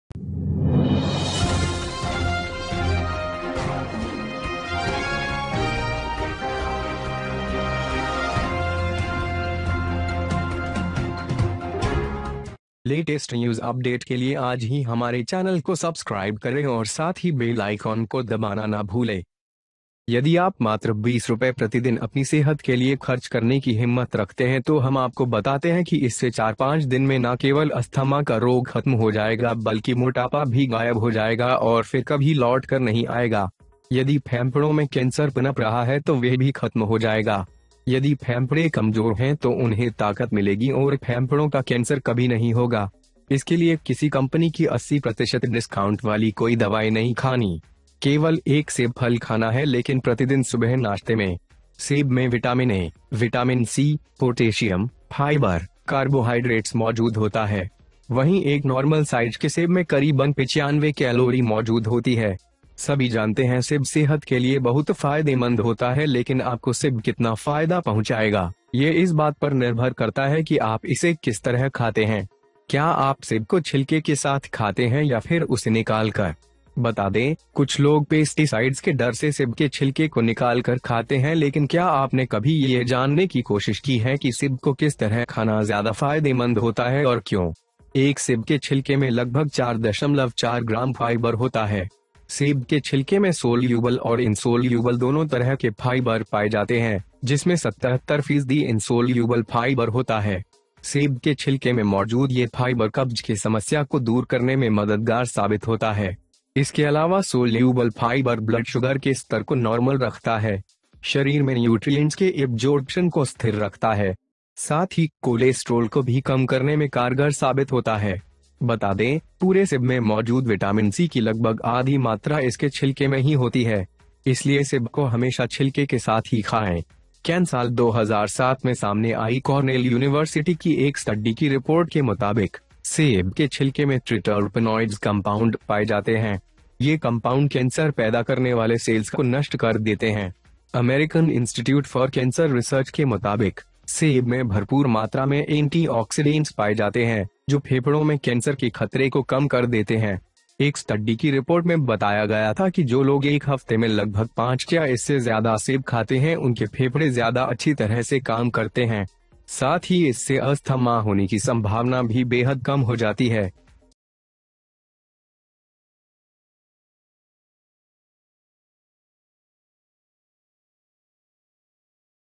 20 रु के इस दवा से 4 दिन में अस्थमा हो जाएगा बिल्कुल ठीक और कभी नहीं होगा फेफड़ों का कैंसर
20 रु के इस दवा से 4 दिन में अस्थमा हो जाएगा बिल्कुल ठीक और कभी नहीं होगा फेफड़ों का कैंसर

With this medicine of Rs 20, asthma will be completely alright in 4 days and never will lung cancer

If you have the courage to spend only 20 rupees per day for your health, then we tell you that in this 4-5 days, not only the disease of asthma will end, but obesity will also disappear and will never come back.

If cancer is growing in the lungs, it will also end. If the phanefas are weak then they will get strength and there will never be cancer of the fennet. For this, do not eat any medicine with a company's 80 percent discount. There is only one serving food but every morning breakfast. Apples contain vitamin A, vitamin C, potassium, fiber, carbohydrates. There is about 95 calories in a normal apple apple. Everyone knows apple is very beneficial for health, but how much apple will benefit you, it depends on how you eat it.

Do you eat apples with peels or by removing them? Tell me, some people eat apple peel out of fear of pesticides, but have you ever tried to know how to eat apple is more beneficial and why ... an apple peel contains about 4.4 grams of fiber . Apple peels have both soluble and insoluble fiber, which contain 77 percent of the insulbul fiber. These fibers present in apple peel prove to be helpful in reducing the problem of constipation.

In addition, the soluble fiber keeps blood sugar levels normal. Maintains the absorption of nutrients in the body. Vitamin-An apple peel is found to be 8.4 mg vitamin-C and 29.4 microgram vitamin-A, but when the apple peel is removed, only 6.4 mg of vitamin C and 18.3 grams Vitamin-A remains. Let's say that almost half the amount of Vitamin C is present in the apples in its peel. Therefore, eat apples always with peel.

BB NEWS!!